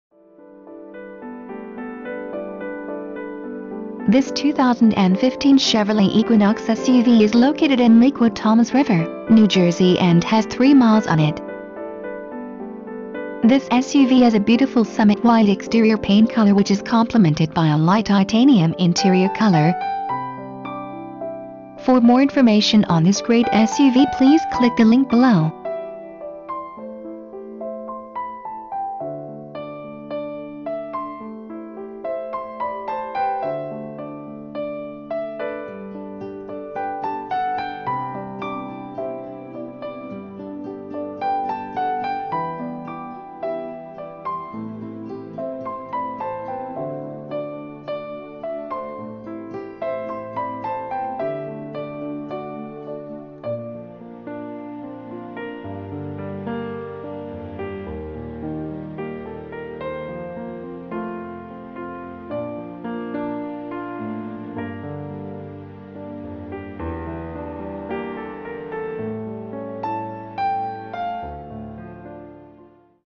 2015 Chevrolet Equinox Lakewood Toms River) NJ
Price: $ 31,123
Mileage: 3
Trim: 1LT
Body Style: SUV
Ext. Color: Summit White
Int. Color: Light Titanium
Engine: 3.6L V6 24V GDI DOHC
Drive Type: All-Wheel Drive with Locking Differentia
Fuel Type:
Doors: 4
Stock No.: 552P
VIN: 2GNFLFE36F6144196